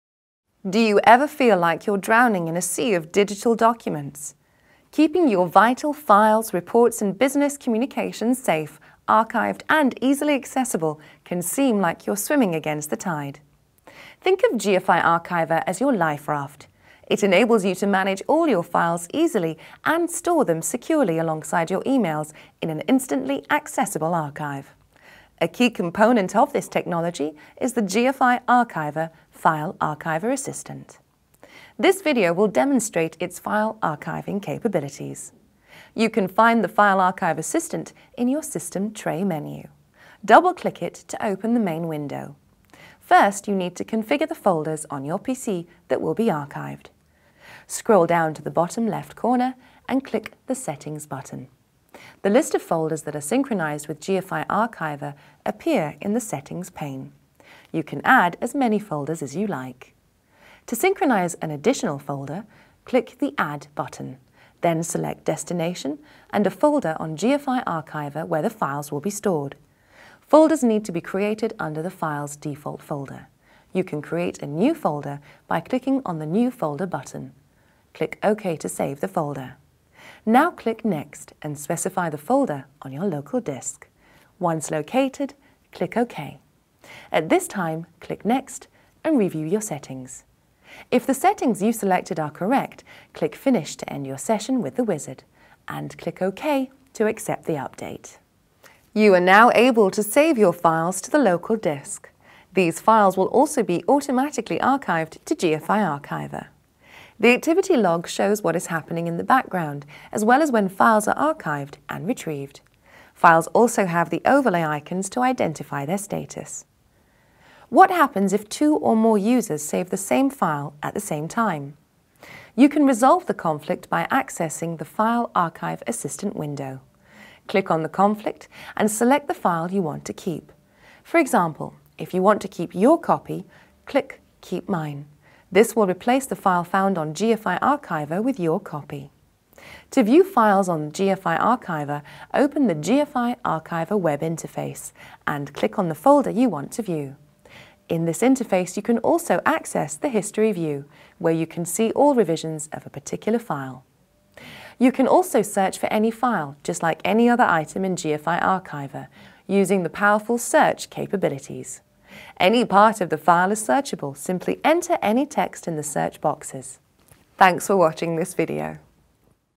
File Archive Assistant Tutorial | GFI Archiver
The File Archive Assistant (FAA) in GFI Archiver enables your users to automatically store copies of important files and folders without user input or interrupting existing workflows.